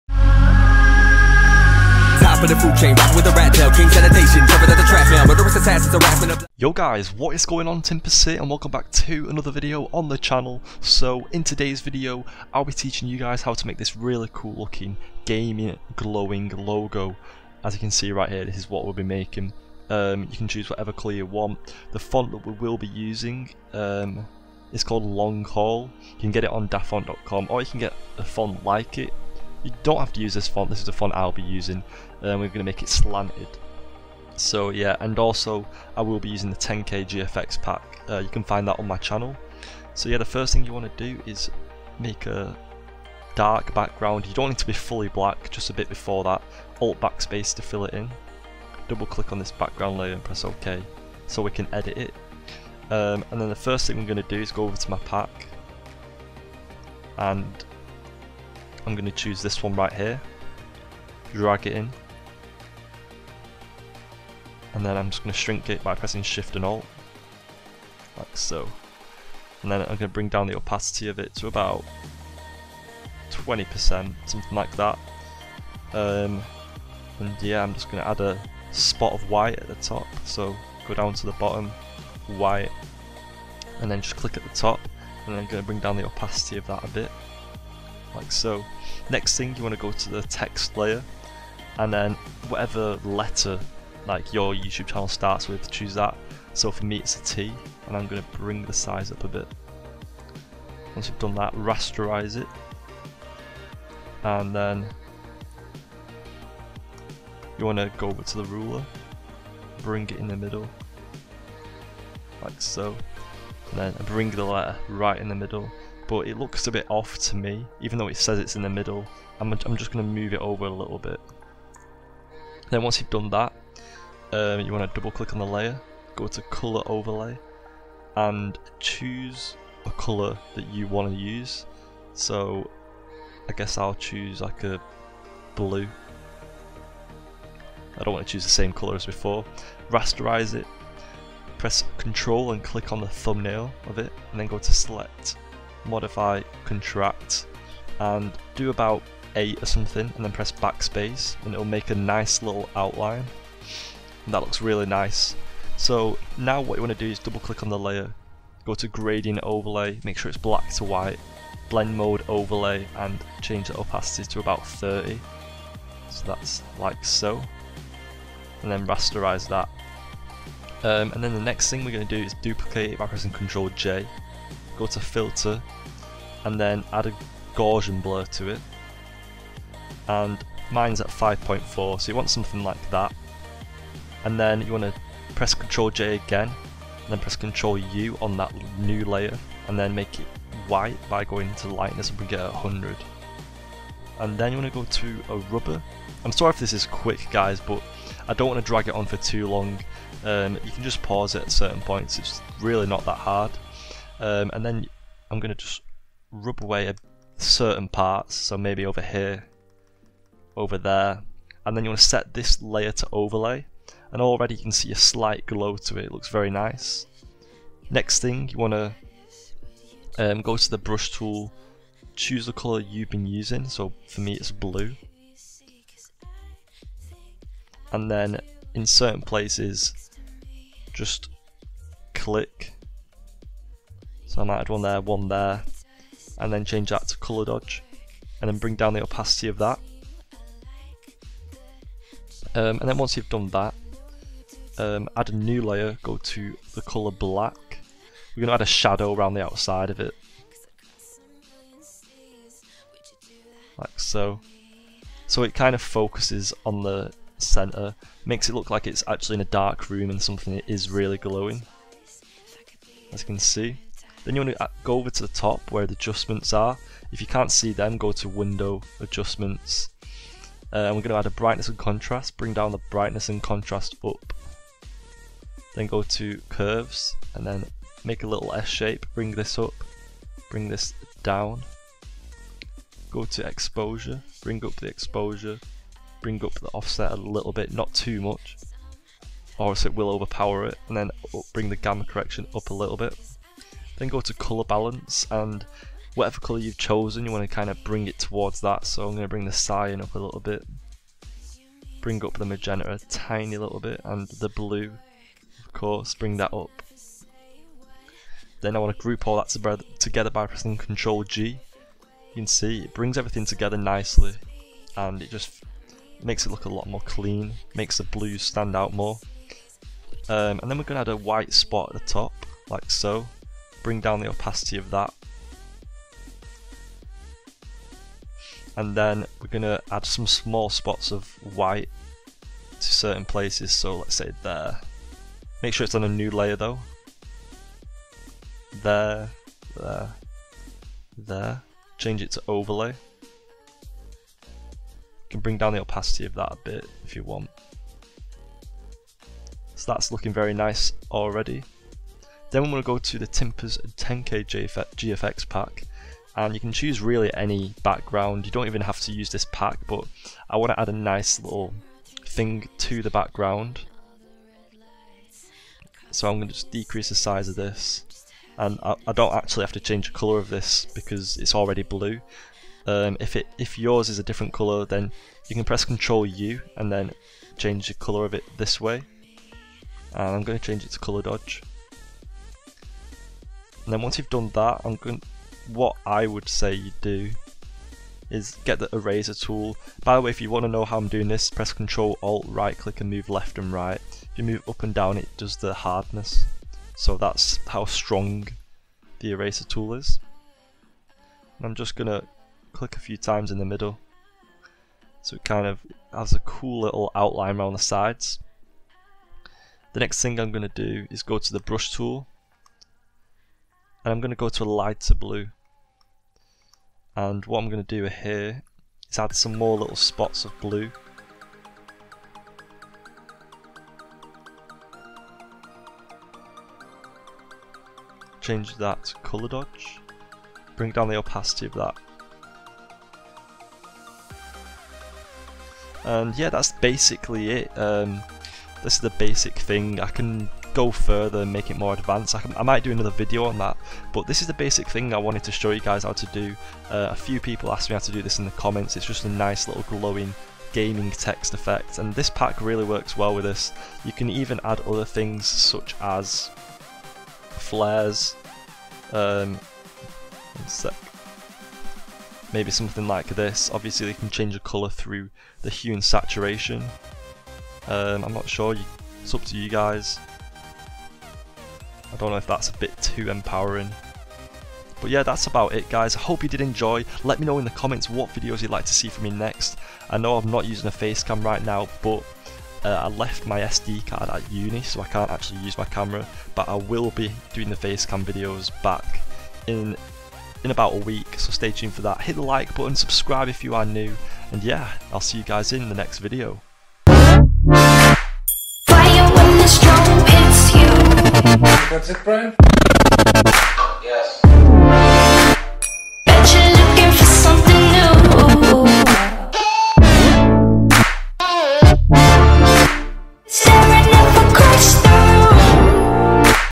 How To Make A Glowing Gaming Logo/Profile Picture in Photoshop 2019
In today's video I'll be teaching you guys how to make a glowing gaming logo in photoshop! Making a glowing gaming logo is very easy!